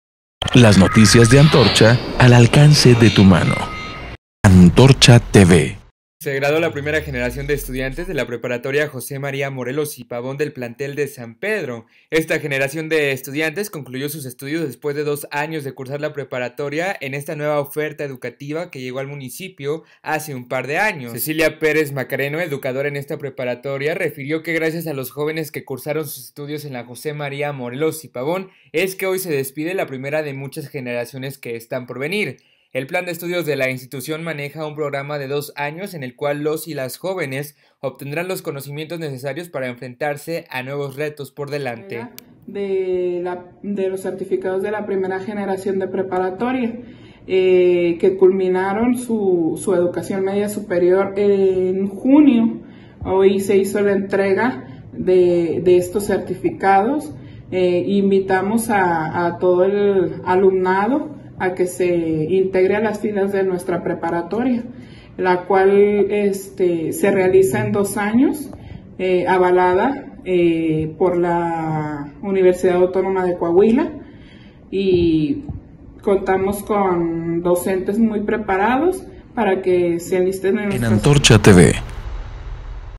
¡Felicidades a la primera generación de la preparatoria José María Morelos y Pavón!
Coahuila 📢📲¡Felicidades a la primera generación de la preparatoria José María Morelos y Pavón!
Una oferta educativa en San Pedro, ideal para prepararte y cursar tu Educación Media Superior.📚